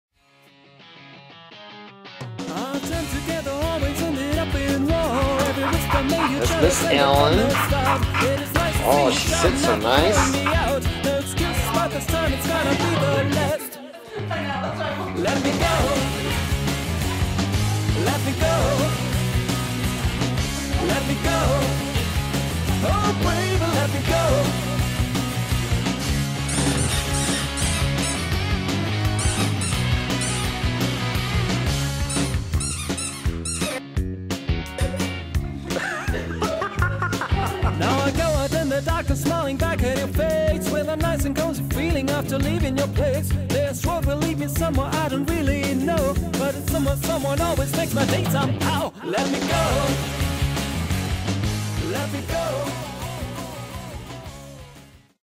Ms Ellen Chako Photoshoot
Here is a great little video of Ms. Ellen a wonderful 2 year old female pit bull. The video and pictures were taken by volunteers of Chako.